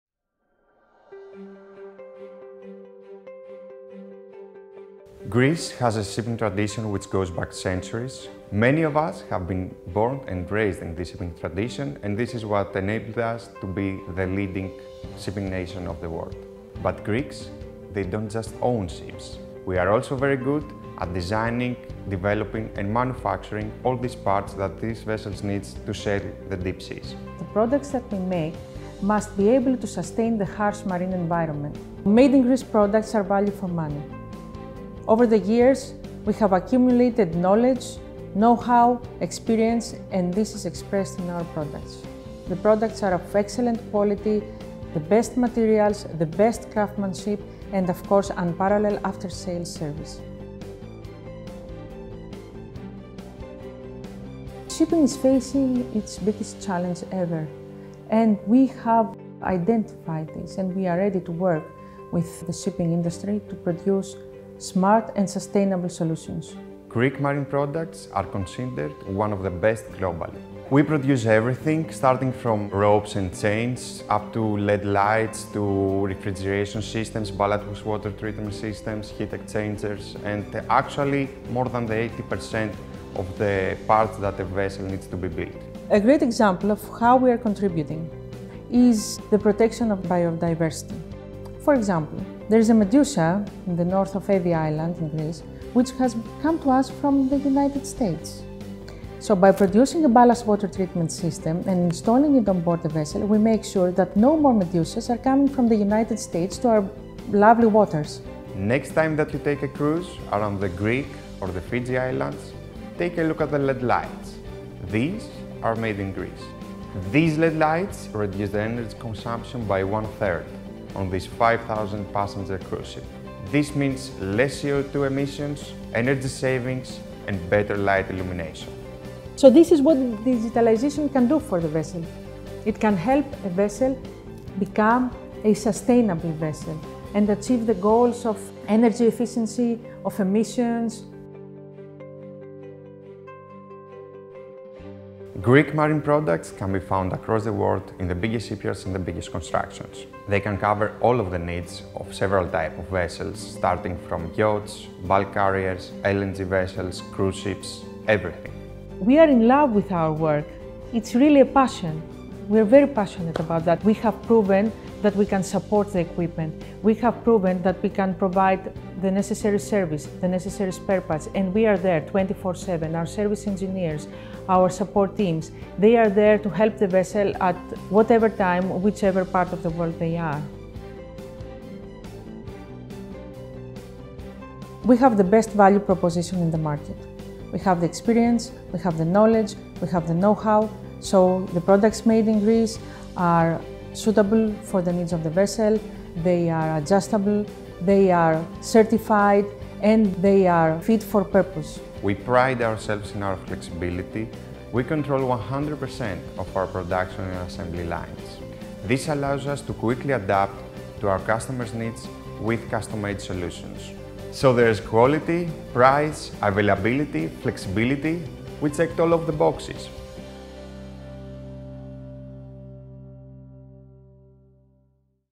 Greek Maritime Technology is Transforming the Shipping Industry
Whether its food, clothes, energy, or even digital appliances, close to 90% of world trade is carried by sea. And it just so happens that Greece tops the ranks as the world’s no. 1 shipowning territory. But Greeks don’t just own ships. In fact, Greece’s maritime technology is considered one of the best globally.

In this video, Helen Polychronopoulou, President of HEMEXPO, and Elias Mallios, Managing Director of Sea Bright, explain how Greece checks all the boxes in building the technology, components, and services that enable these vessels to run and be competitive in the first place.